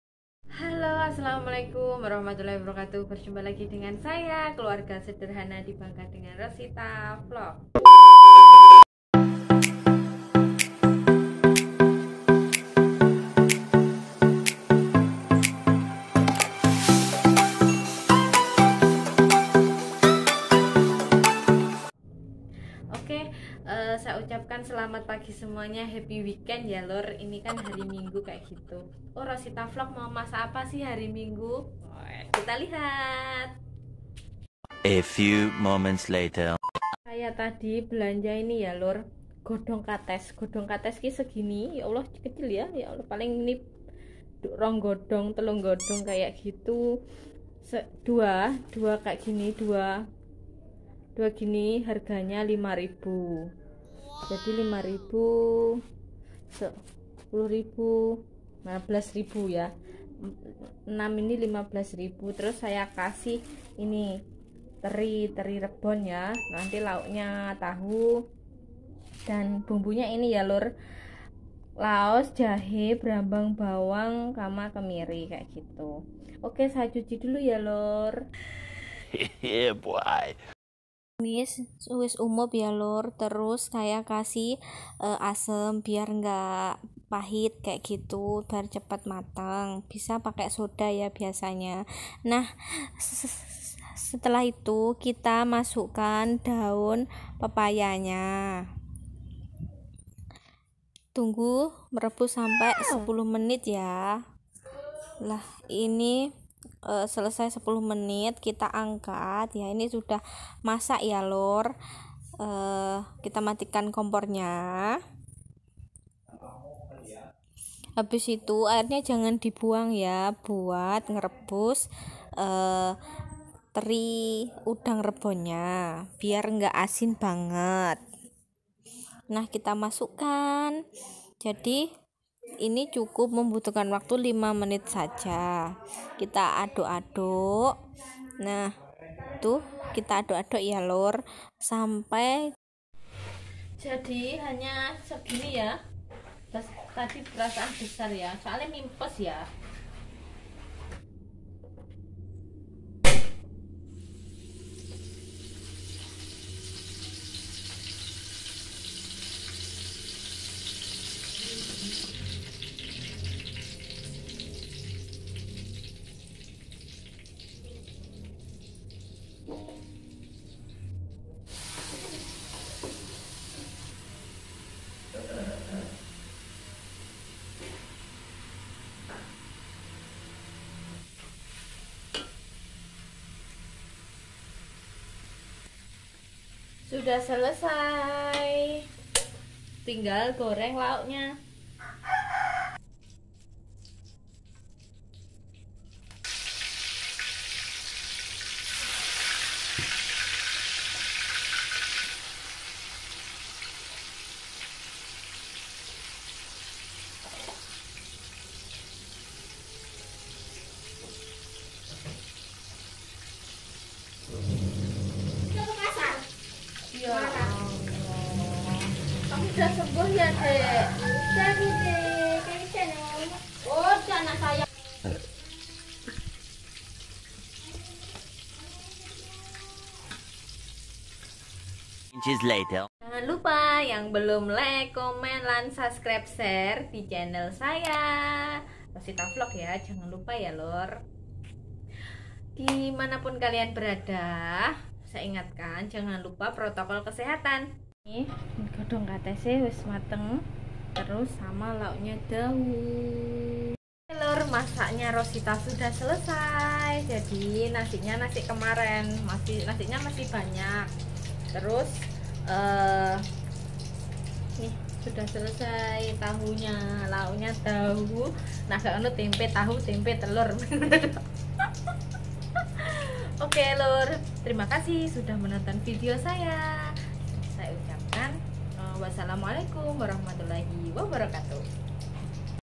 CHEF DESI NUMIS GODONG KATES LAUK TAHU GORENG#CANDA TAHU GORENG😄
Daun pepaya
B.putih
B.merah
Jahe& laos
Cabe kecil
Garam
Udang rebon
Kecap secukupnya